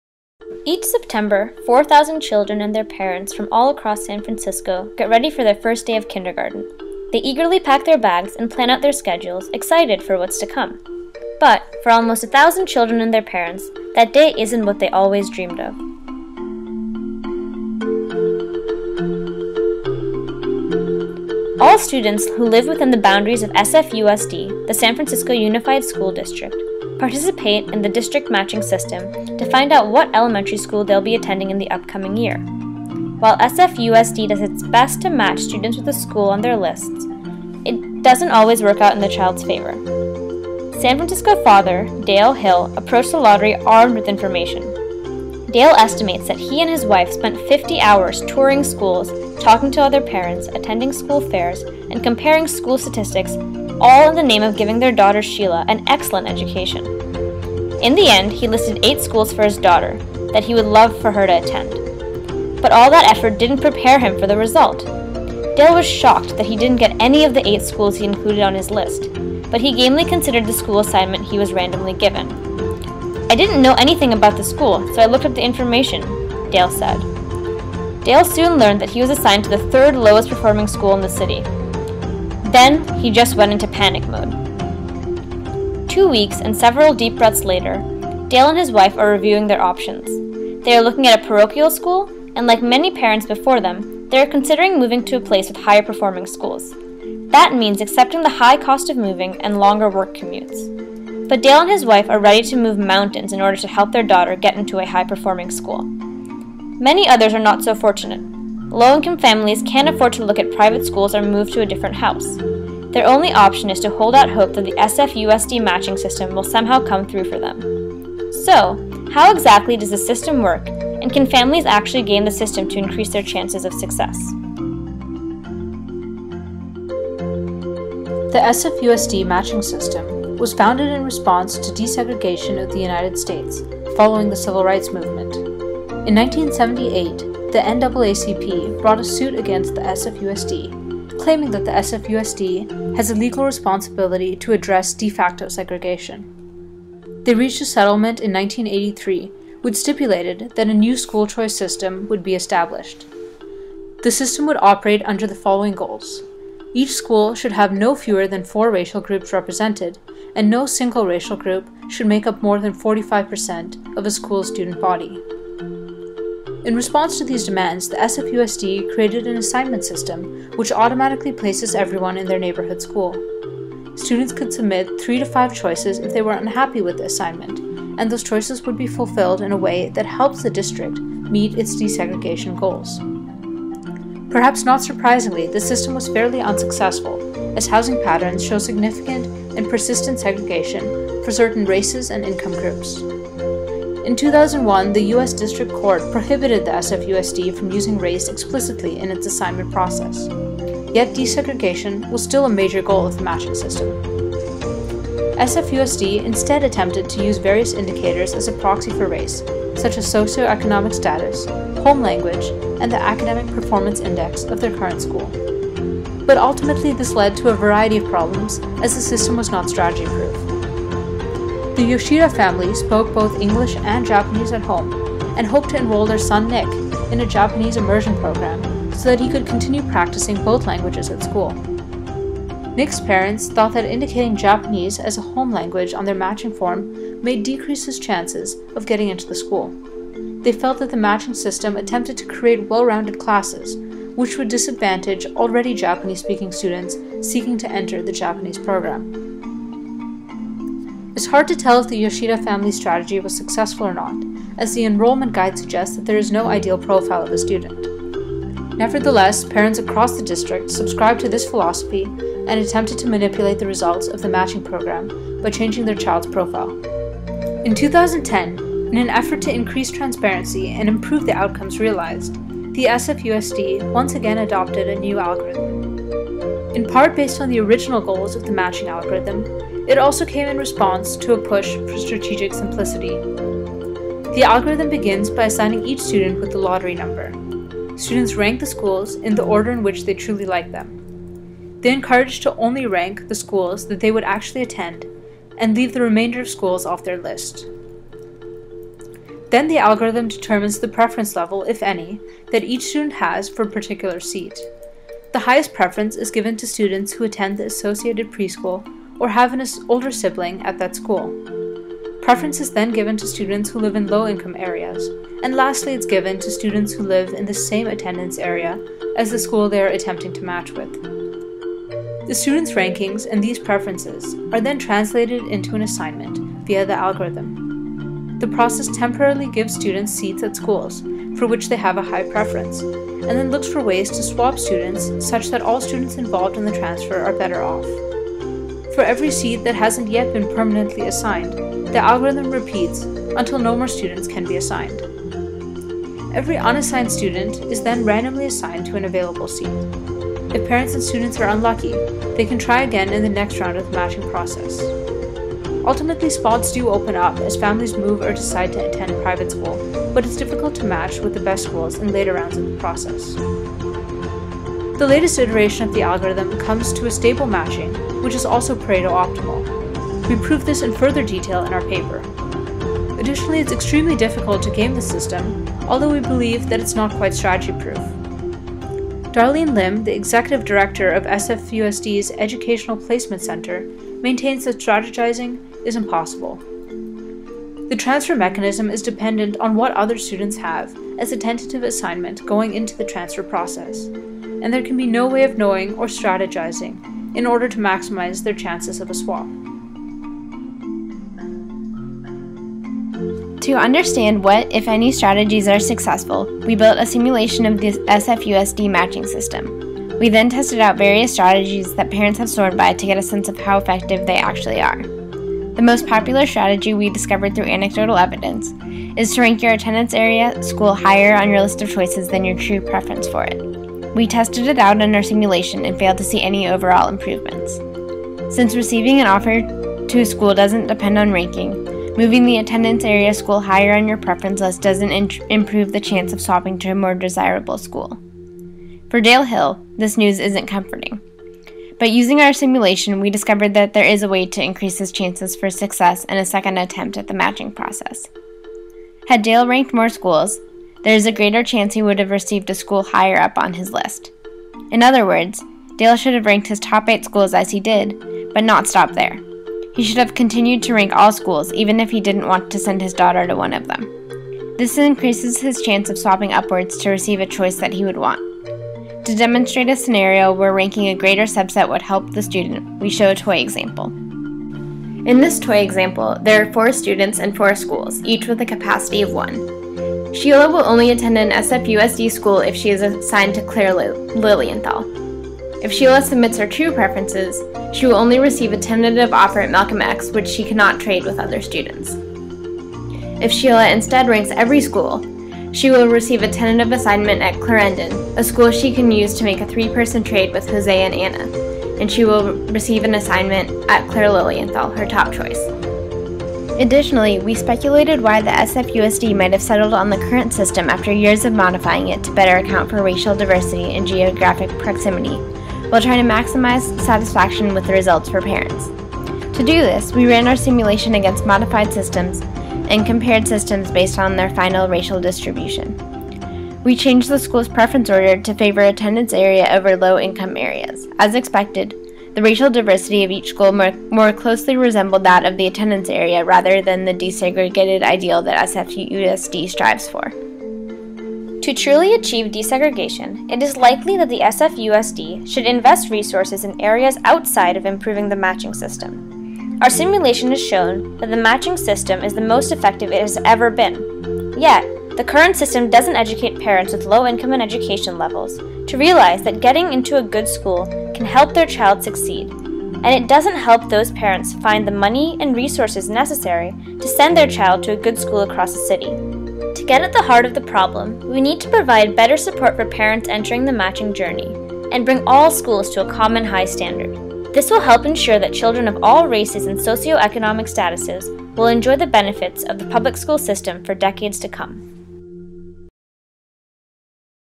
Anything For My Child: Incentives in the San Francisco Elementary School Matching Process
Stanford University CS269i Final Project
By: Ambika Acharya, Tara Balakrishnan, Sarah Rosston